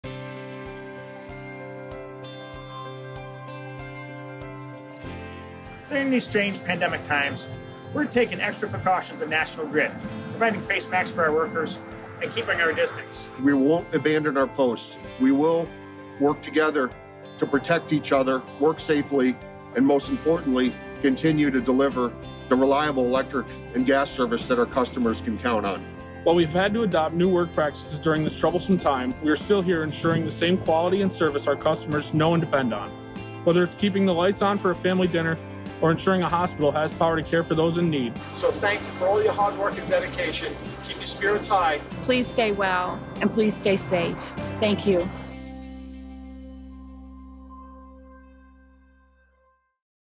National Grid's Field Safety During COVID-19
Please know that we are following CDC safety precautions in the wake of COVID-19 as we continue to deliver essential services to our customers and communities.Stay strong and stay safe.